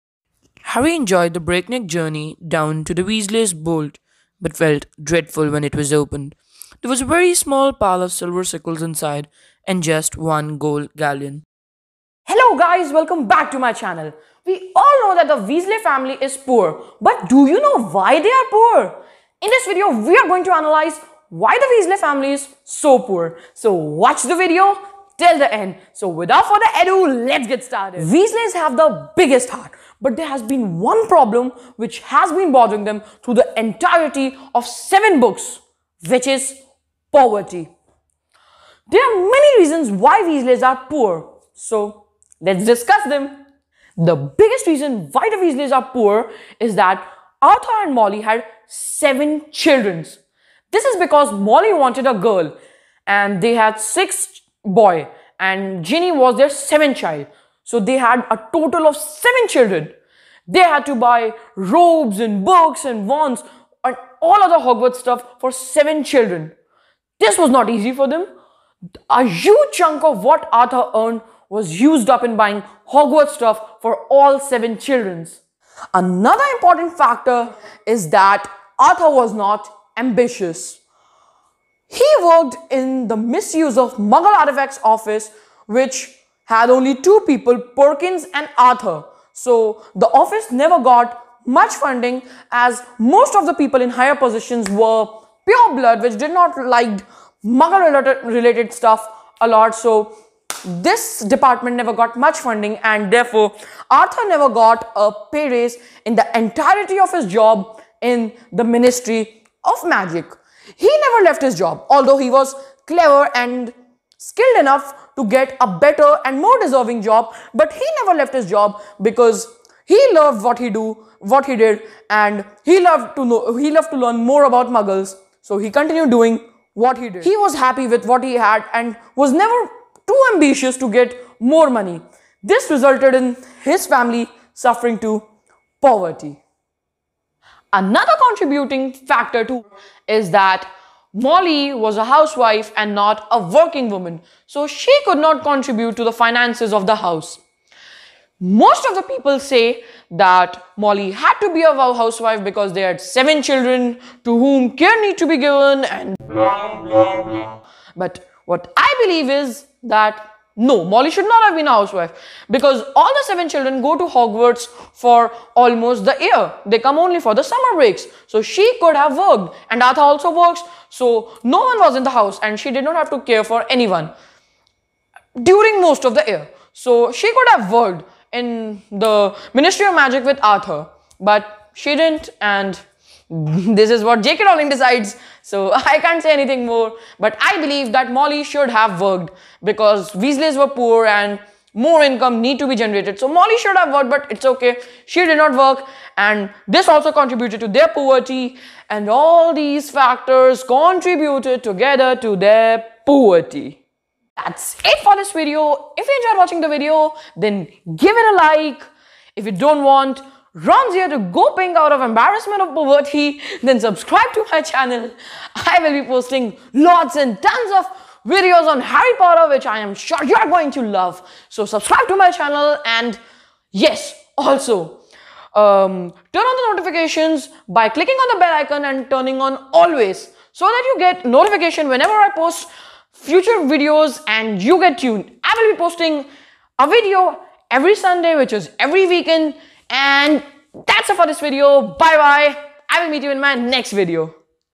Why Were The Weasleys So poor?
Hello Guys,
We all know that the weasley's are poor, but do you know why they are poor?
In this video I will analyse why Weasley's are poor!
Thanks